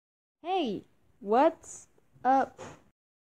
Animating the mouth
FlipaClip - Anime dude
This took so much longer than I thought! I ACCTUALLY finished about 2 weeks ago but I'm really busy with school right now that it took me till now to upload, sorry to keep everybody waiting!